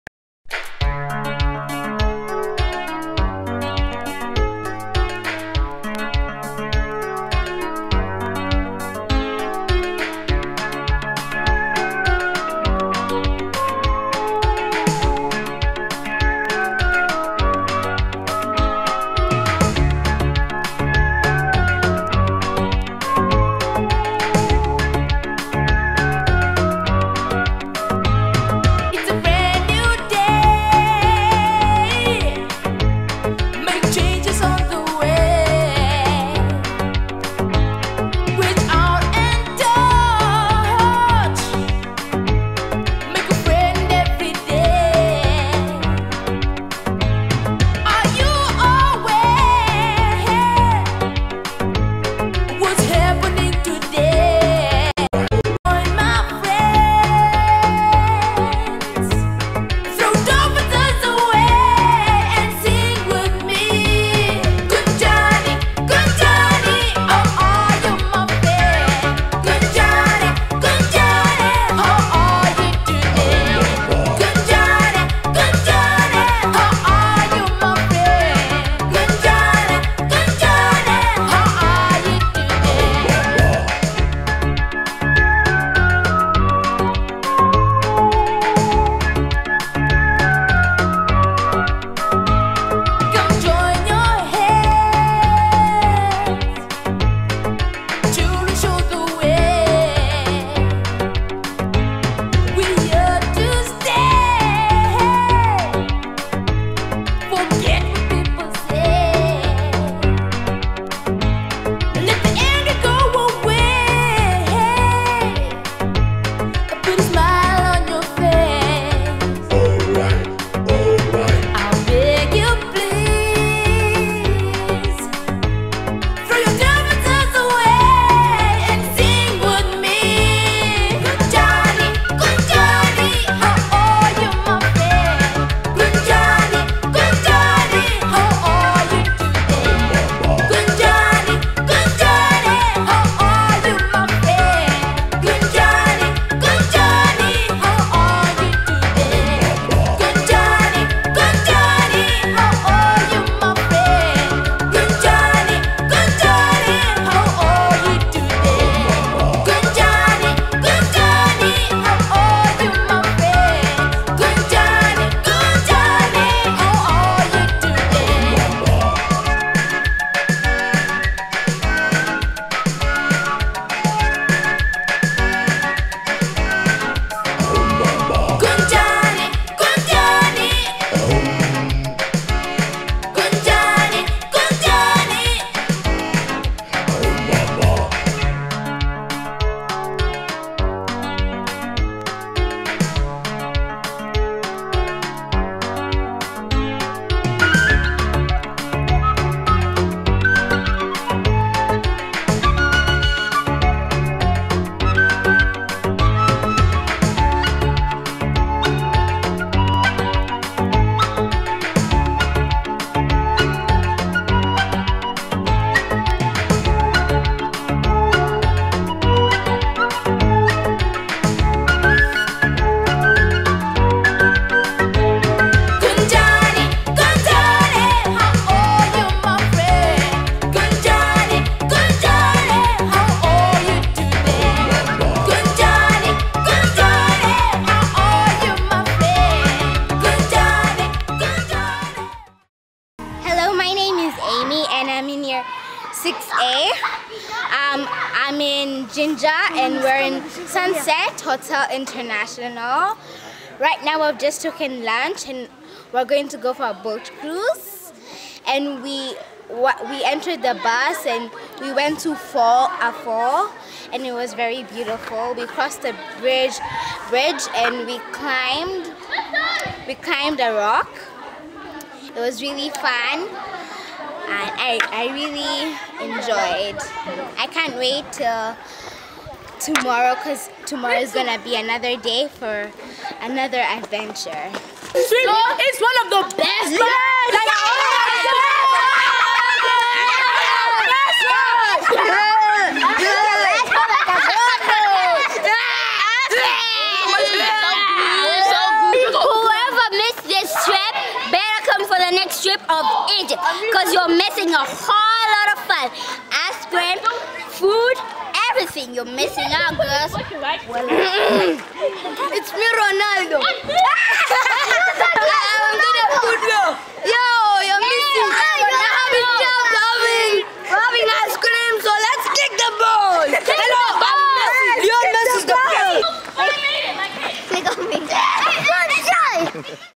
Galaxy International School Uganda TRIP to Eastern Uganda 2017
Places , River Nile Jinja, Sipi Falls in Kapchorwa, Rock Painting in Kumi